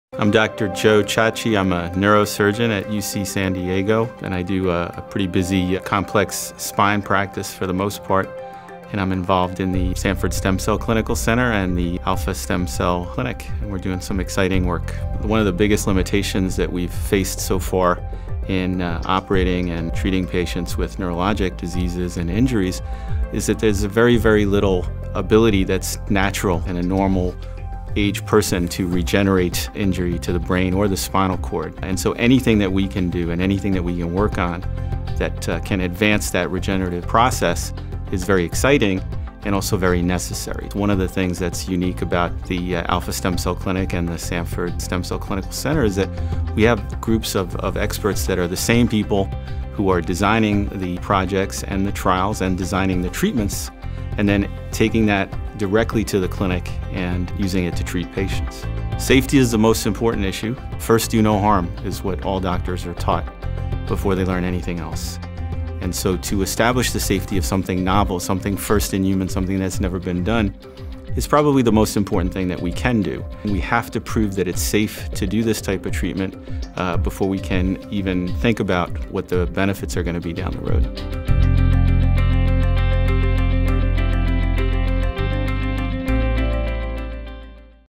Meet Joseph Ciacci, MD: UC San Diego CIRM Alpha Stem Cell Clinic
In this “Meet the Experts” series of videos for UC San Diego CIRM Alpha Stem Cell Clinic, Joseph Ciacci, MD, explains the limitations and challenges of recovering from brain or spinal cord injury. He also discusses the need for safely conducted research through a team approach.

Dr. Ciacci is the clinic’s trial expert for neurosurgical technique and the principal investigator for the clinical trial on spinal cord injury. He has extensive experience in neuro-oncology and is a professor in the Department of Surgery at UC San Diego School of Medicine.

Learn more about the UC San Diego CIRM Alpha Stem Cell Clinic:
health.ucsd.edu/alphaclinic